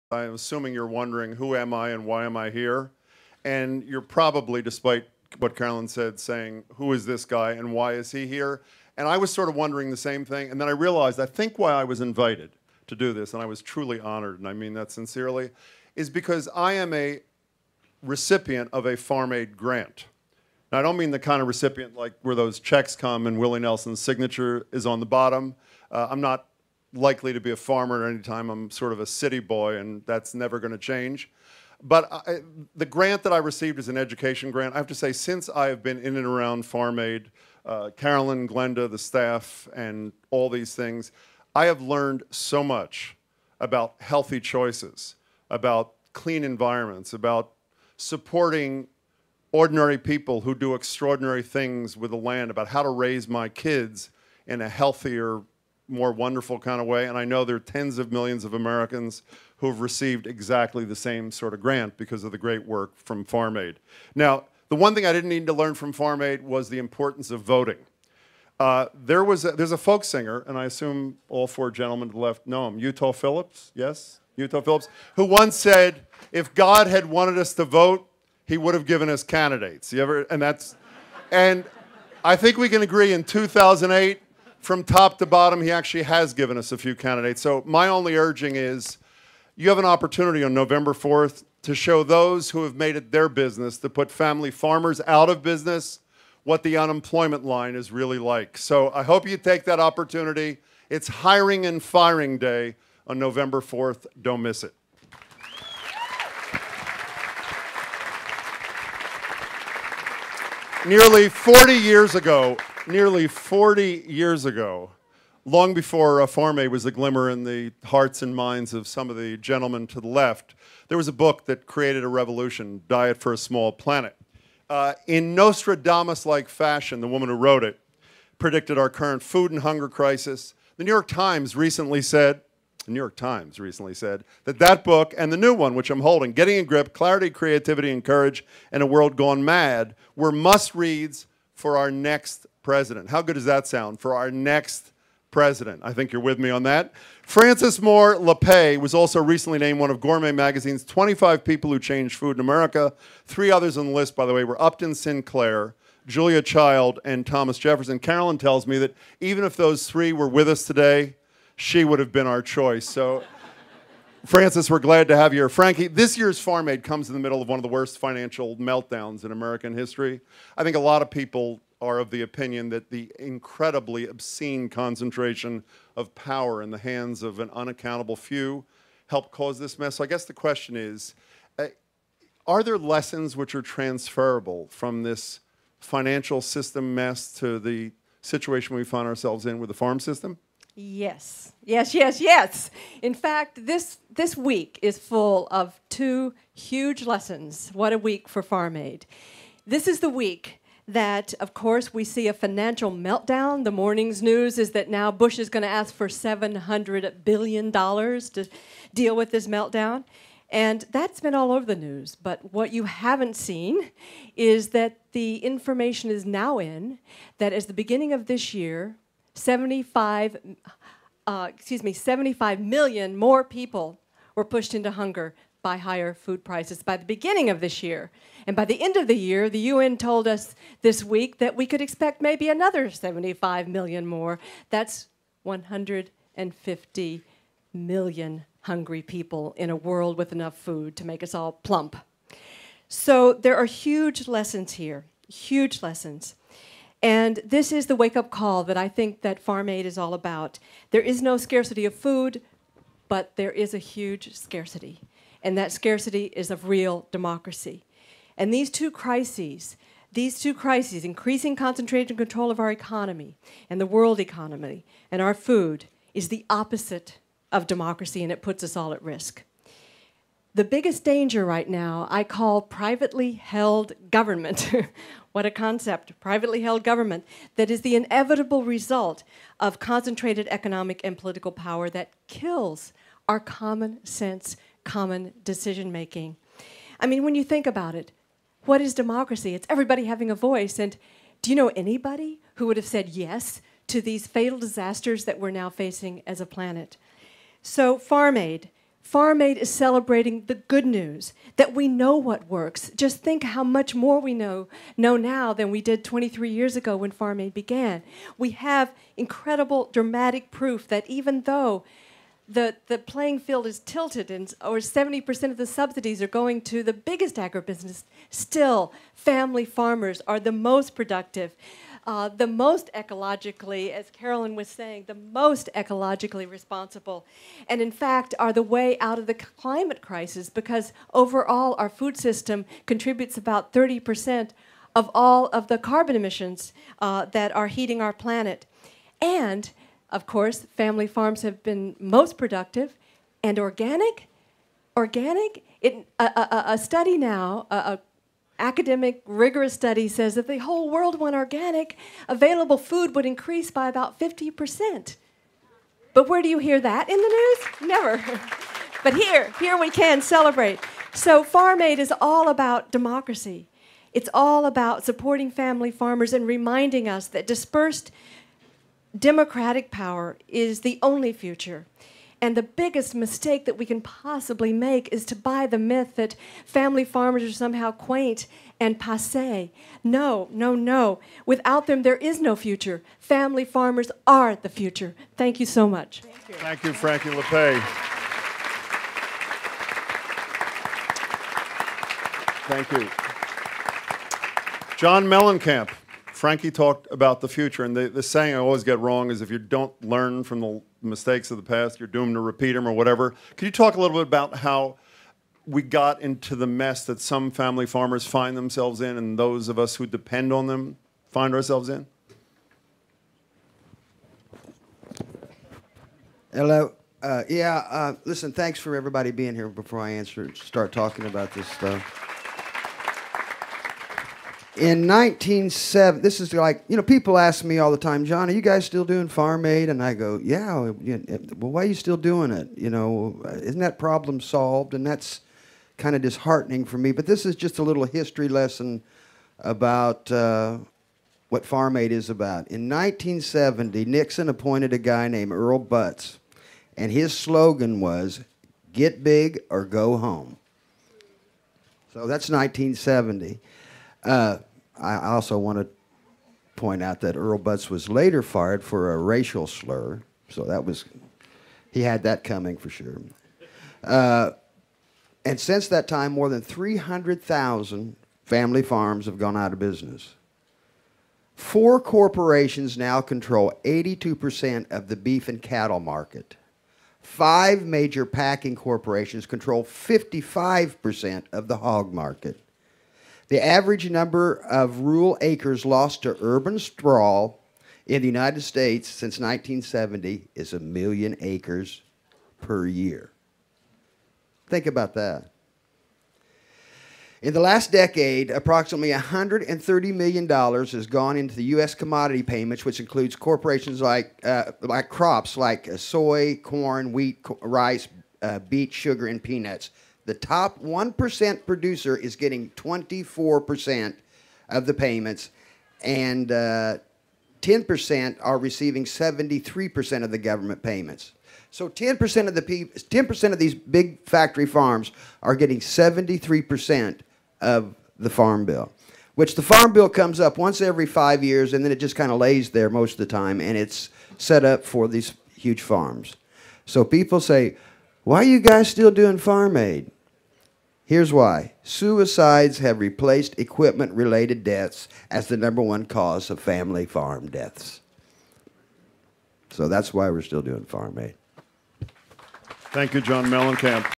Farm Aid 2008 - Press Event (part 2 of 5)
This is the press event held the morning of Farm Aid's 2008 concert in Mansfield, Massachusetts. Willie Nelson, Neil Young, John Mellencamp, and Dave Matthews speak about the importance of family farmers for our food, the environment, the economy, and in our lives.

NECN host Jim Braude serves as the M.C. of the event and Farm Aid Executive Director Carolyn Mugar also speaks, along with Massachusetts Governor Deval Patrick, Francis Moore Lappé, Brenna Chase, and Duwan Grant. Appearing on stage are the musical performers The Elms, Will Daley, One Flew South, Grace Potter and the Nocturnals, moe., Arlo Guthrie, Jamey Johnson, Nation Beat, and Jakob Dylan.